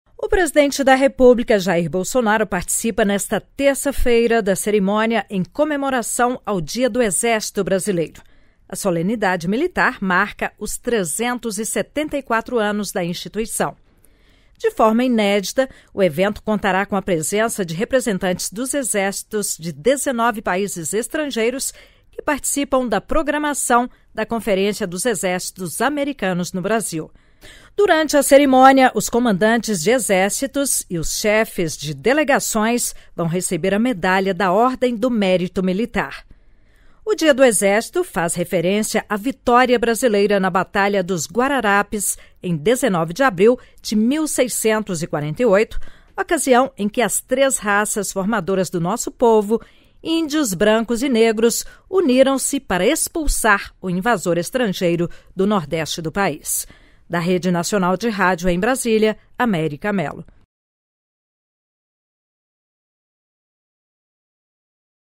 19/04/2022 - PRESIDENTE JAIR BOLSONARO PARTICIPA DO DIA DO EXÉRCITO
Data faz referência à vitória brasileira na Batalha dos Guararapes, em 19 de abril de 1648.

O presidente da República, Jair Bolsonaro, participa nesta terça-feira (19) da cerimônia em comemoração ao Dia do Exército Brasileiro.

A solenidade militar marca os 374 anos da instituição.

De forma inédita, o evento contará com a presença de representantes dos Exércitos de 19 países estrangeiros, que participam da programação da Conferência dos Exércitos Americanos no Brasil.

Durante a cerimônia, os Comandantes de Exércitos e os chefes de delegação vão receber a medalha da Ordem do Mérito Militar.

O Dia do Exército faz referência à vitória brasileira na Batalha dos Guararapes, em 19 de abril de 1648, ocasião em que as três raças formadoras do nosso povo – índios, brancos e negros – uniram-se para expulsar o invasor estrangeiro do Nordeste do País.

registrado em